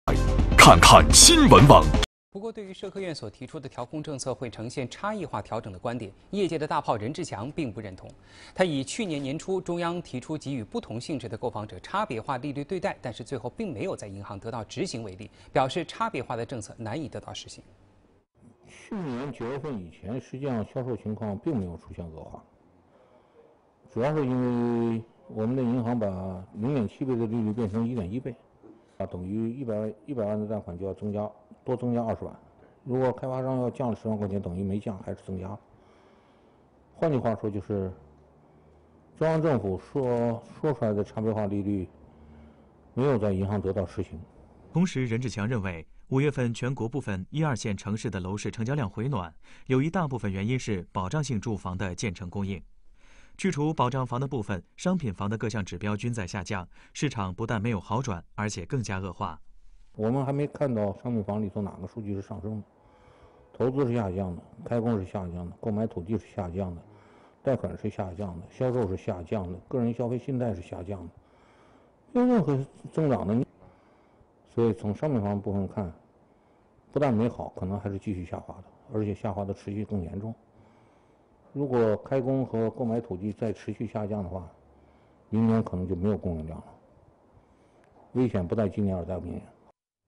任志强：差别化政策将遇执行难.mp4
不过，对于社科院提出的调控政策会呈现差异化调整的观点，业界的"大炮"任志强并不认同。他以去年年初中央提出给予不同性质的购房者差别化利率对待，但最后并没有在银行得到执行为例，表示差别化的政策难以得到实行。
任志强，地产商，北京市政协委员，北京市华远地产股份有限公司董事长，同时兼任北京市商业银行监事、新华人寿保险公司董事。自1993年起改组成立北京市华远房地产股份有限公司，并担任董事长兼总经理，创建"华远"品牌，在房地产界具有极高的知名度。2011年4月其透露正式接到上级的免职命令，退去华远集团董事长一职，但仍担任华远地产上市公司董事长。在"财经年会2012"分论坛上，任志强与住建部展开争论，被指"吃了肉还骂娘"。2012年4月，媒体报道称任志强公开表示一年半后房价暴涨，针对此说法，其在微博上回应这不是他的原话与本意。